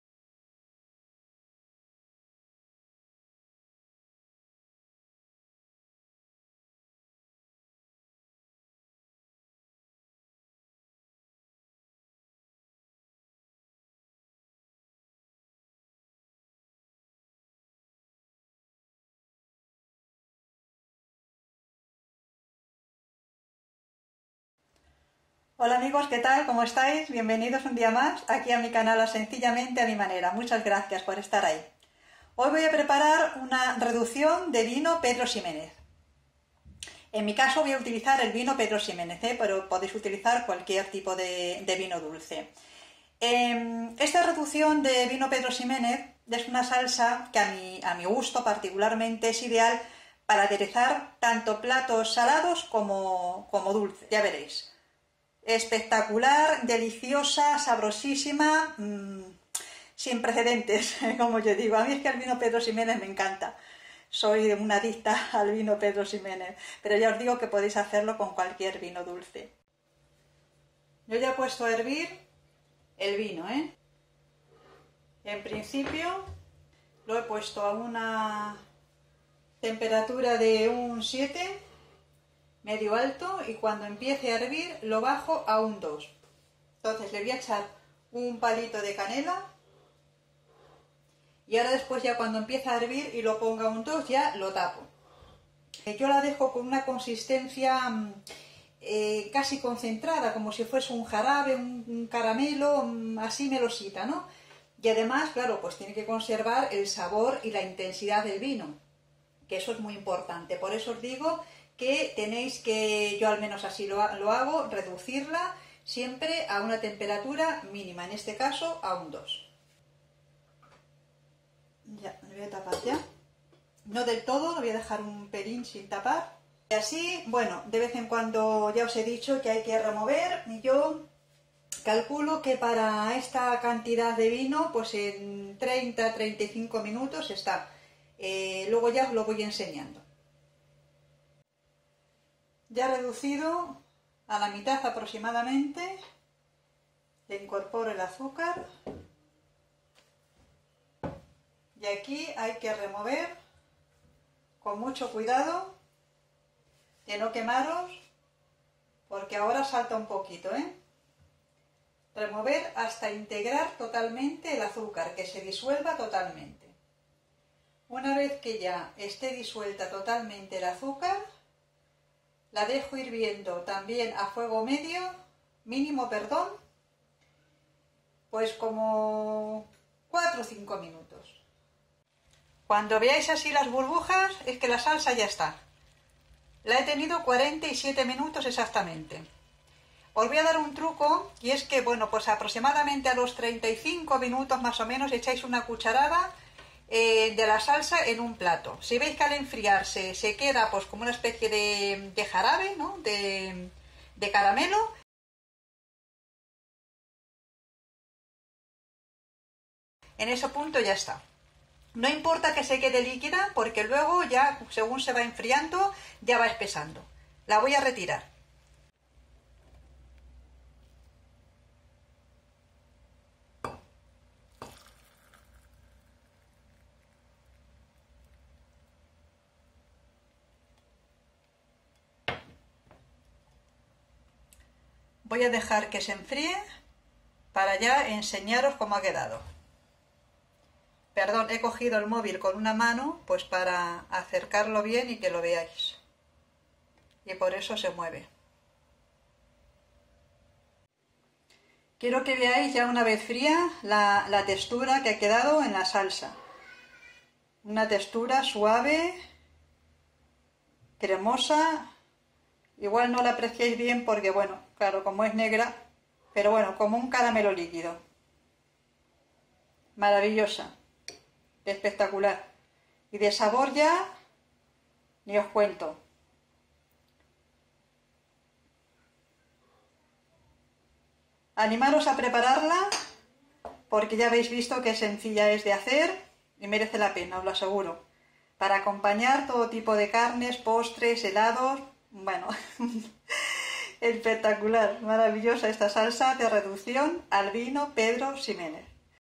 REDUCCIÒN DE CUALQUIER TIPO DE VINO. EN MI CASO VINO DULCE PEDRO XIMÉNEZ
Os dejo esta riquísima y espectacular  receta de reducción de vino dulce, en mi caso Pedro Ximénez. Espero que os guste.
Un cordial saludo.

INGREDIENTES:

250g de vino Pedro Ximénez.
Entre 70 u 80g. de azúcar (al gusto) Podéis utilizar azúcar blanquilla o morena.
Una rama de canela (opcional)